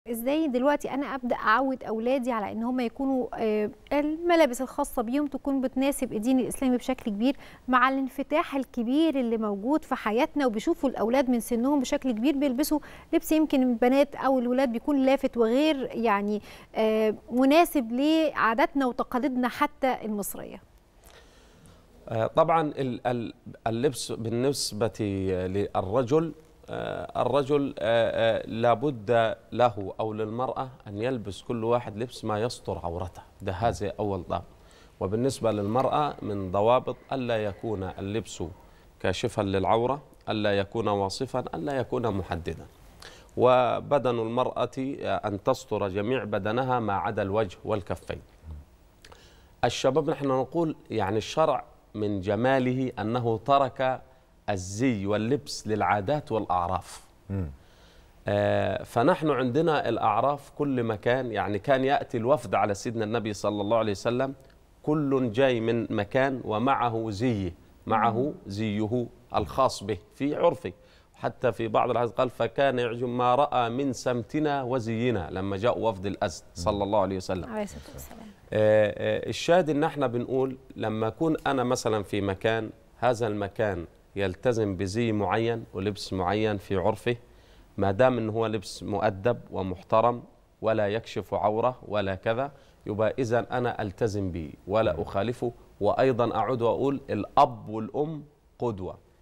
إرشادات لتربية الأبناء تربية سليمة ؟ تعرف عليها مع الواعظ بمركز الأزهر العالمي للفتوى الإلكترونية
إرشادات لتربية الأبناء تربية سليمة؟ تعرف عليها مع الواعظ بمركز الأزهر العالمي للفتوى الإلكترونية د. إسماعيل محمد عبدالحي